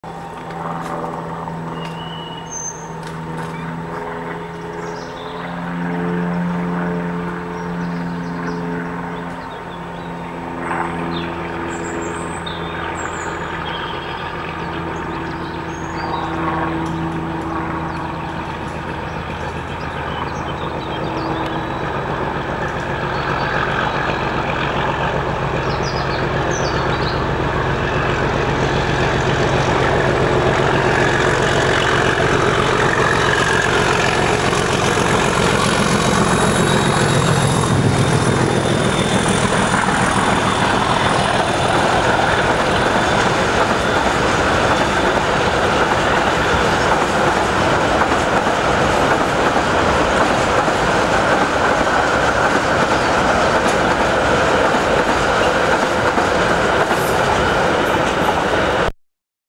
37798 5th May 1995 Oxford Hinksey Yard
Mainline Blue booked traction ,  37798  heads a binliner train South of Oxford.